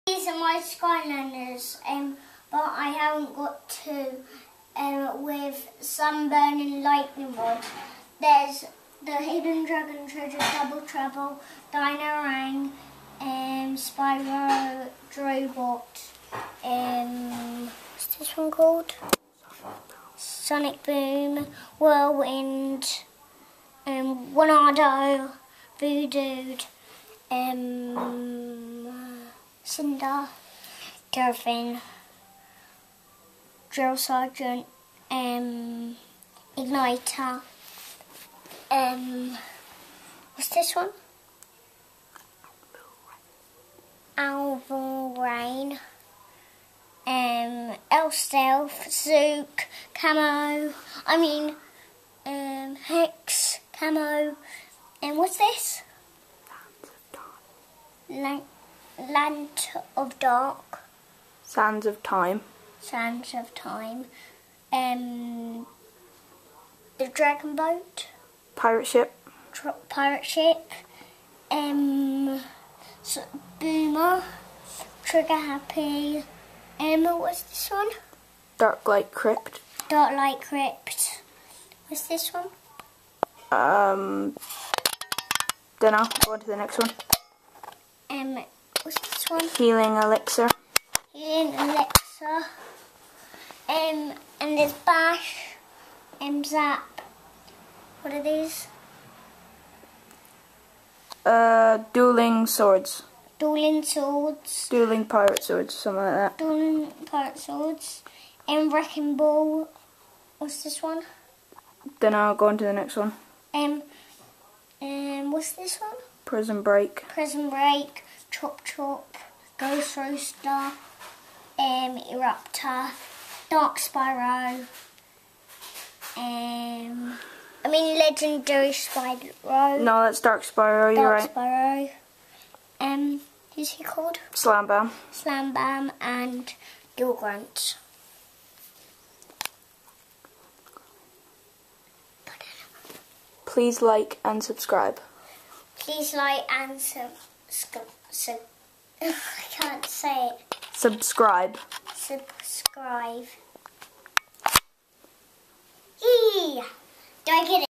cute 3 year old kid shows off his collection of skylanders!
he knows all the names!